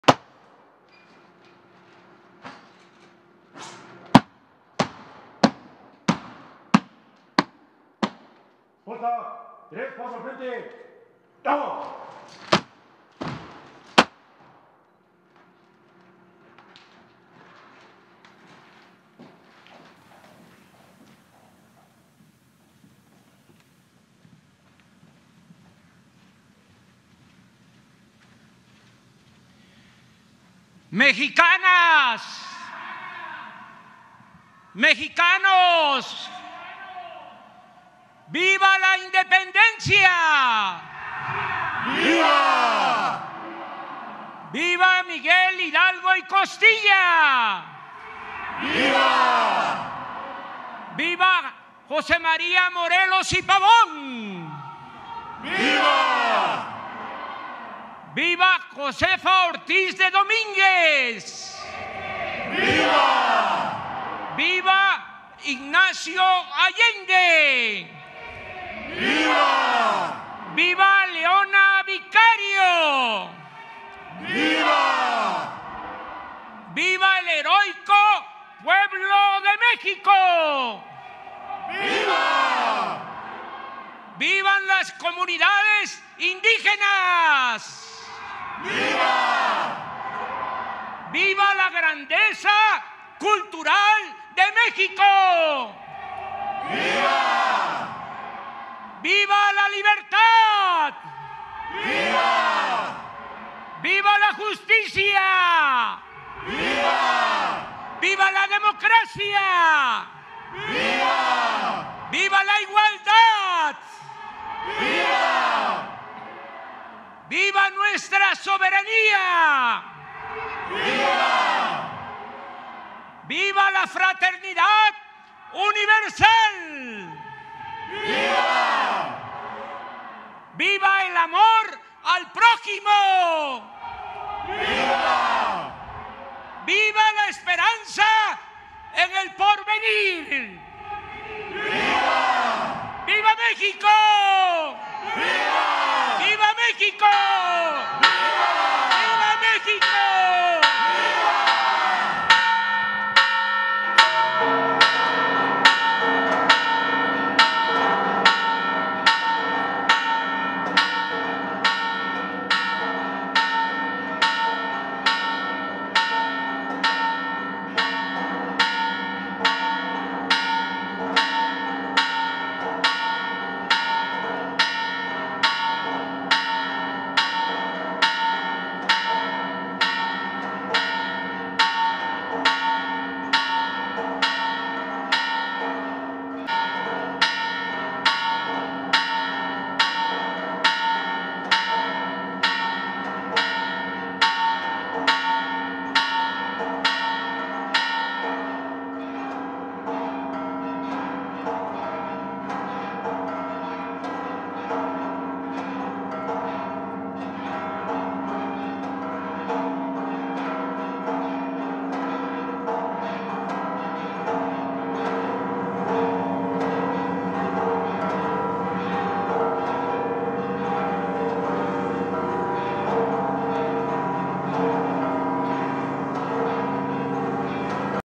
Grito de Independencia 2020
🇲🇽 No tengo palabras para describir la ceremonia de esta noche en la conmemoración de nuestra Independencia.
La mirada franca del Presidente, la bandera ondeando, la llama de la esperanza, los fuegos artificiales y el popurrí de nuestra música. Conmovedor, histórico, patriótico.
Sin duda, una ceremonia atípica llena de nostalgia por los estragos de la pandemia, pero a fin de cuentas, maravillosa y llena de simbolismos.
Cuéntenme, qué les pareció?